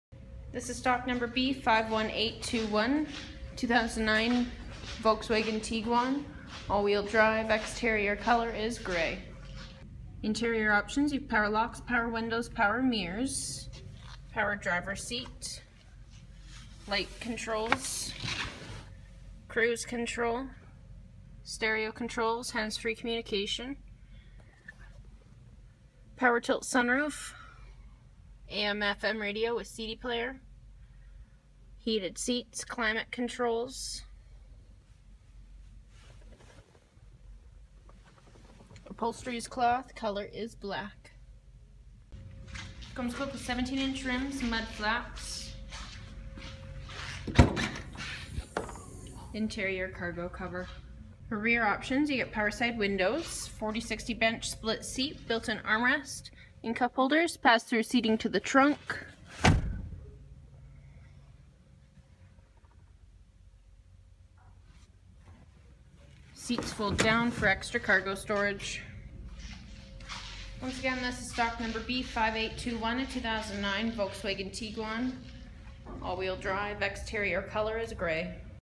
2009 Volkswagen Tiguan 4dr Auto Trendline 4Motion
This is a 2009 Volkswagen Tiguan 4dr Auto Trendline 4Motion with 6-Speed A/T transmission Gray[X9X9,Alpine Grey Metallic] color and Black interior color.  This video is recorded and uploaded by cDemo Mobile Inspector